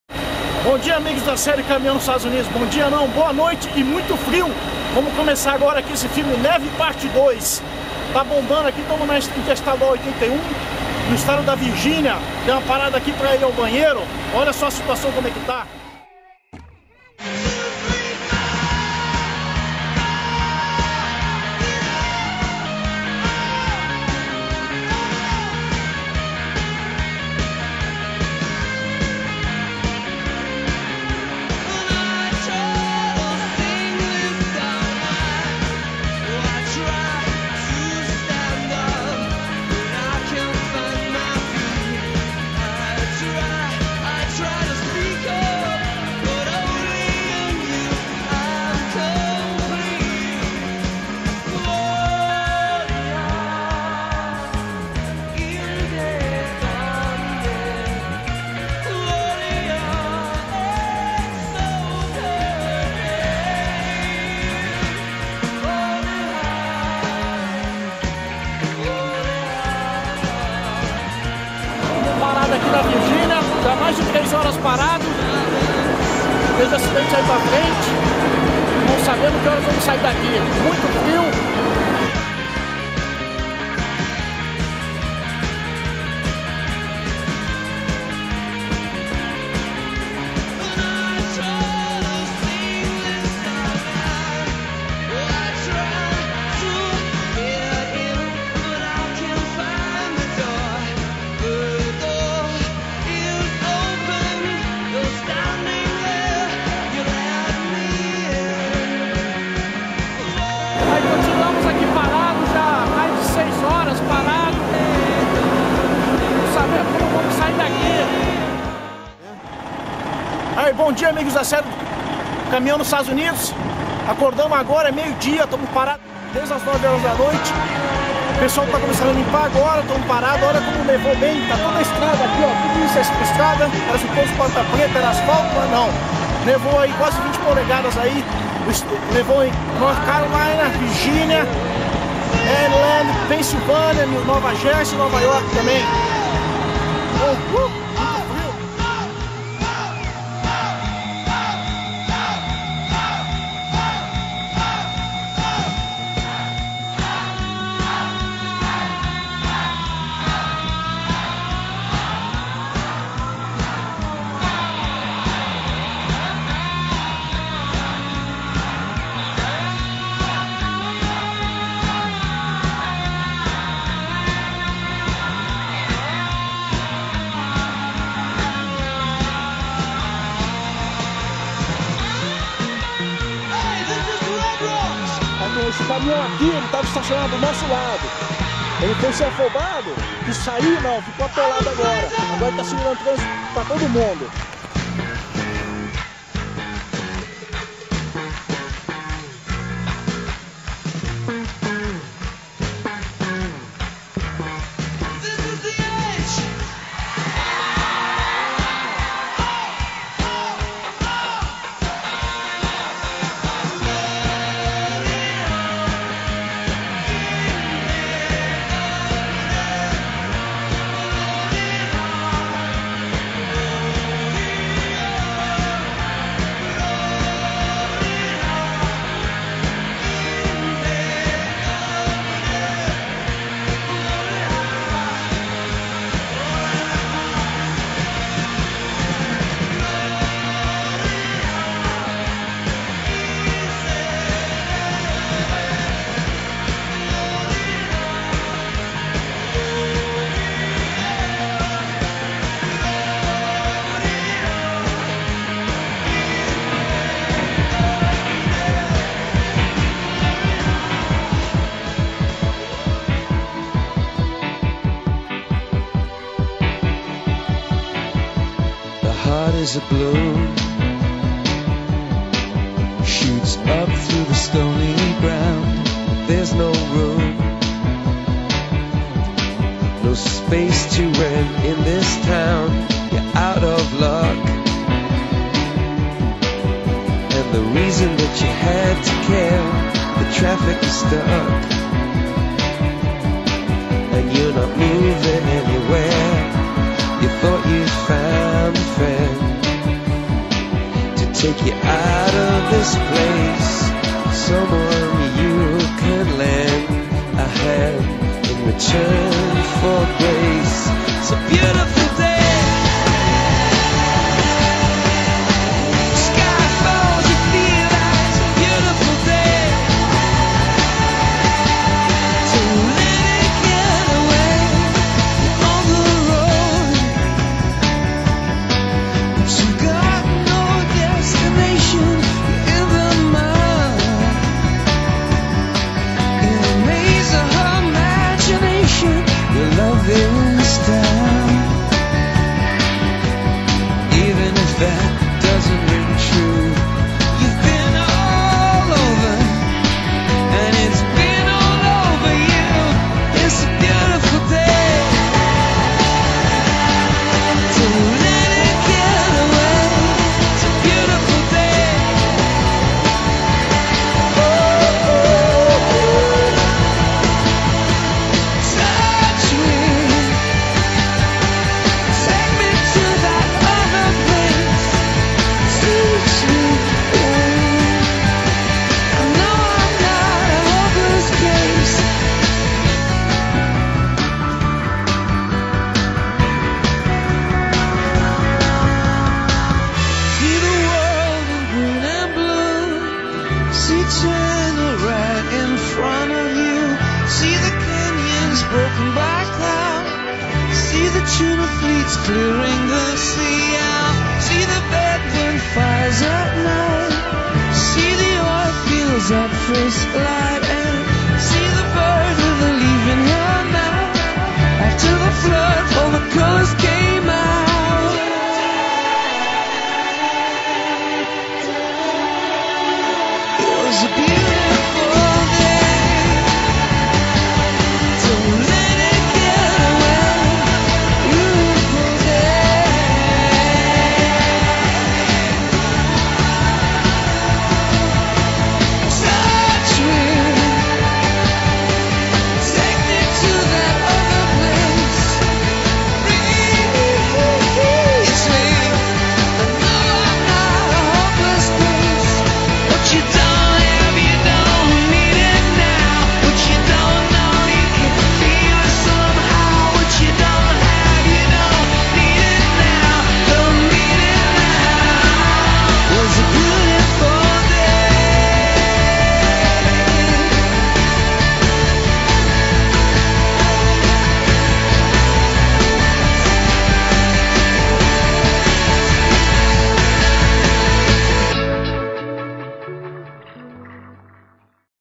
Viagem de caminhao na neve Part II Caminhao nos Estados Unidos HD
Dirigindo na neve. A tempestade de neve que atingiu a costa leste americana no final de semana antes do natal.